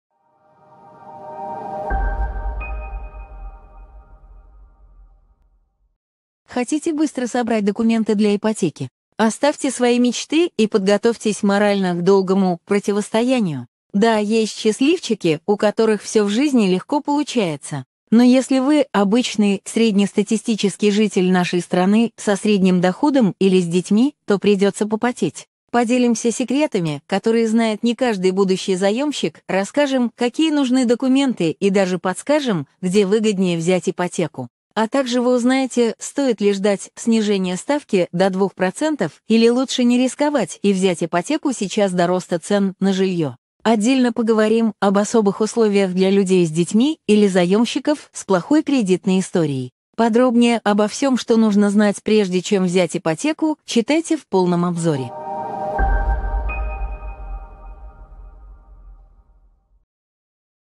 Как быстро собрать документы для ипотеки?
При оформлении ипотеки мы сталкиваемся с многими трудностями. О том, как быстро и правильно собрать документы и многие другие секреты, которые знает не каждый заемщик, читайте в полном обзоре